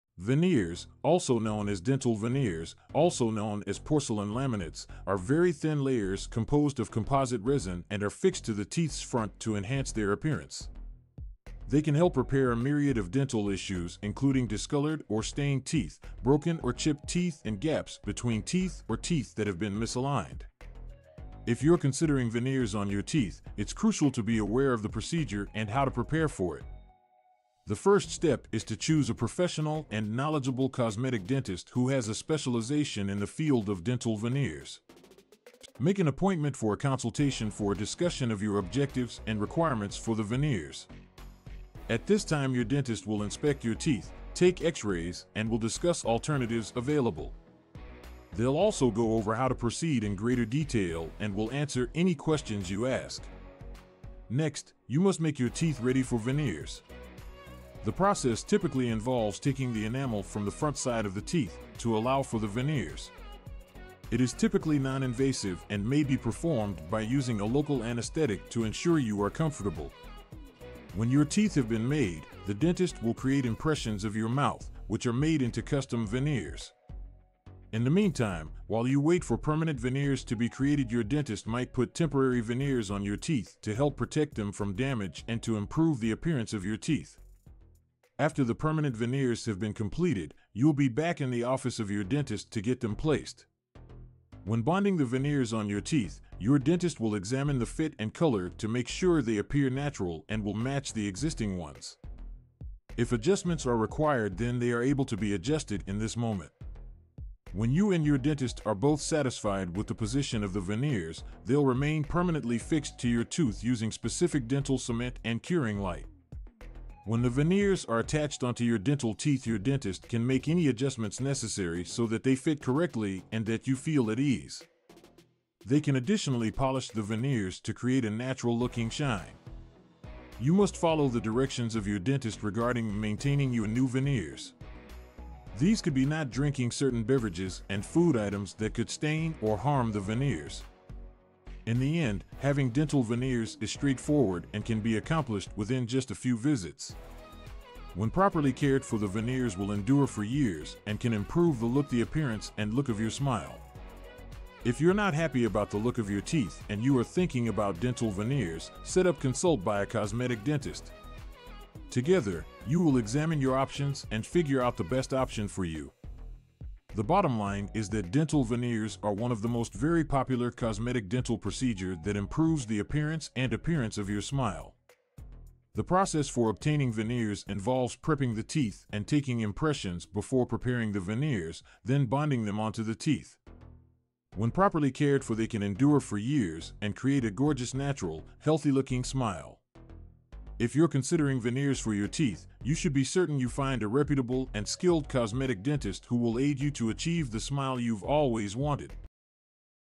Transform Your Smile with Veneers - How Do Veneers Work and What Problems Can They Fix?
Telegram Veneers, also known as dental laminates, are thin layers of composite resin that are placed on the front of teeth to improve their appearance. They are a popular cosmetic dental treatment that can help fix a variety of issues, such as discoloration, chips, and gaps between teeth. Veneers are custom-made to match the color and shape of your natural teeth, providing a natural-looking and long-lasting solution. They are a less invasive option compared to other cosmetic treatments, and can give you a brighter, more confident smile. With proper care, veneers can last for many years, making them a great investment in your dental health.